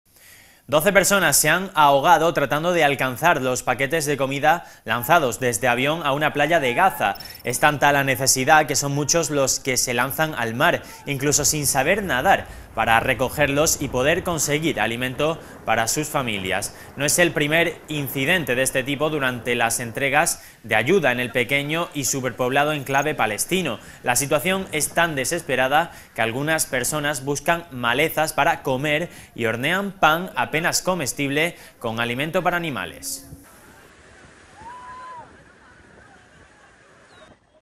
🔴Noticia - Mueren 12 palestinos ahogados al tratar de coger comida lanzada al mar
Doce personas se han ahogado tratando de alcanzar los paquetes de comida lanzados desde avión a una playa de Gaza. Es tanta la necesidad, que son muchos los que se lanzan al mar, incluso sin saber nadar, para recogerlos y poder conseguir alimento para sus familias. No es el primer incidente de este tipo durante las entregas de ayuda en el pequeño y superpoblado enclave palestino. La situación es tan desesperada que algunas personas buscan malezas para comer y hornean pan, apenas comestible, con alimento para animales.
English:

Twelve people have drowned trying to reach food packages dropped by plane onto a Gaza beach. The need is so great that many people jump into the sea, even without knowing how to swim, to collect them and be able to get food for their families. It is not the first incident of its kind during aid deliveries in the small, overcrowded Palestinian enclave. The situation is so desperate that some people forage for weeds to eat and bake barely edible bread with animal feed.

Live streaming